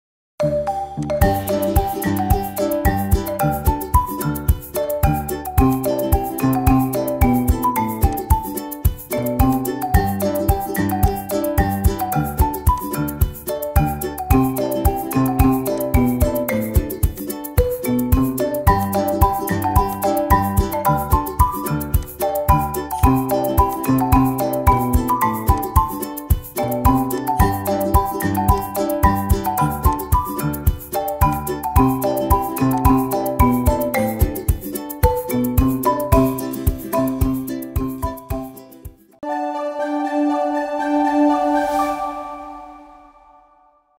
Music Review Game Template
This Music themed PowerPoint allows you to change the questions and answers! The animations are added, the links are set, and the fonts and backgrounds are already made. All you have to do is add the questions and answers!